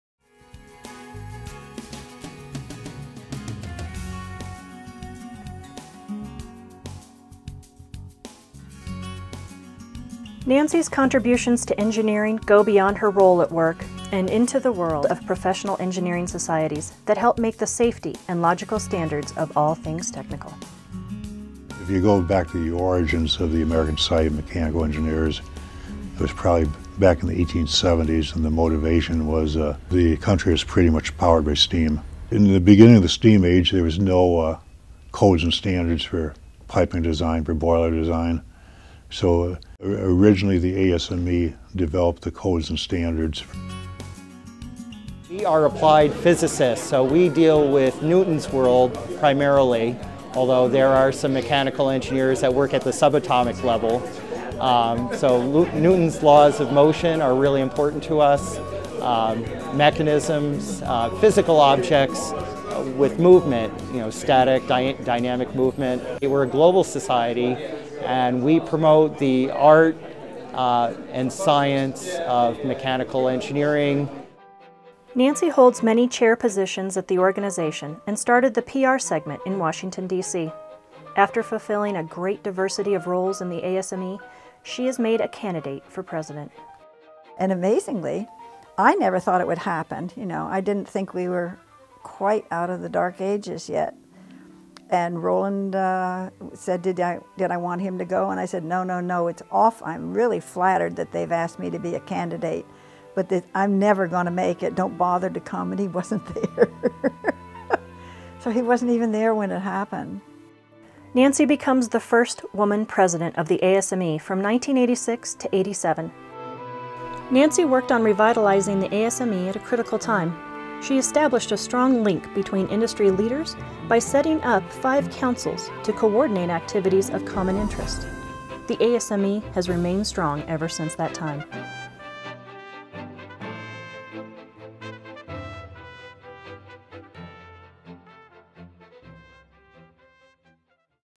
ASME's First Woman President
American Society of Mechanical Engineers welcomed it's first woman president Nancy Fitzroy in the 1980s. She is an example of some of the great engineers of her generation. This excerpt also discusses the origin and mission of the ASME.